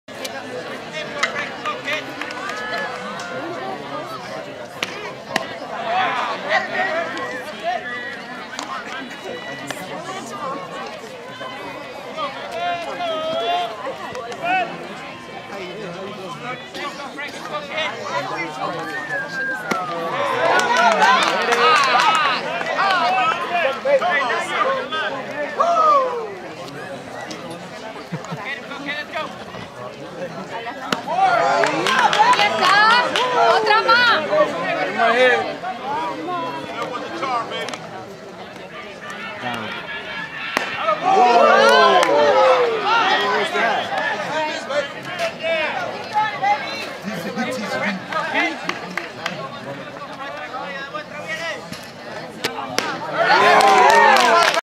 Foul tip catch for out
Rob keeps his pitcher on track to throw strikes and get the batter to foul tip the 3rd strike for the out vs Osceola high 2/26/2016